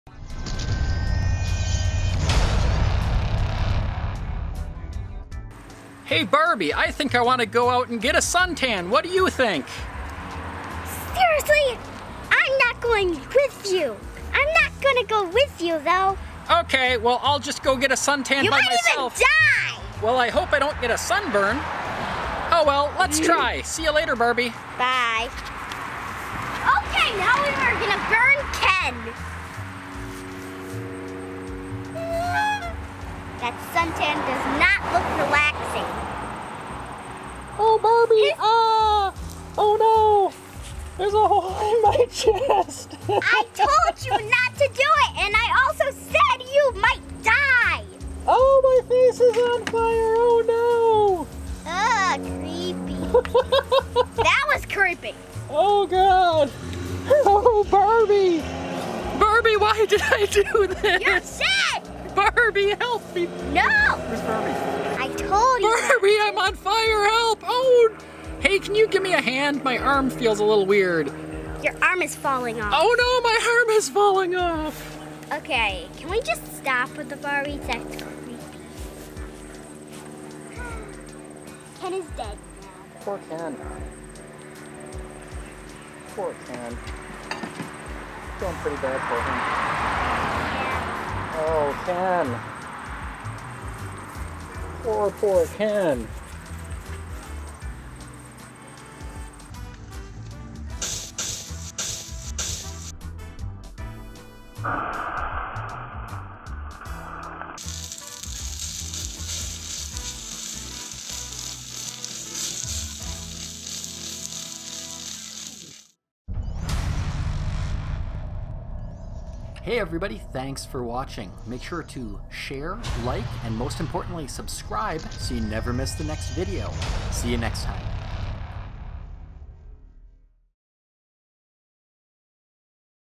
Solar Death Ray vs Barbie and Ken
Barbie and Ken take on the Solar Death Ray! Hope they packed their sun tan lotion!
Our Solar Death Ray is built from the front screen of an old projection TV, but fresnel lenses of smaller sizes are sold as inexpensive magnifiers. Keep in mind that even the small ones are pretty fun for scientific experimenting!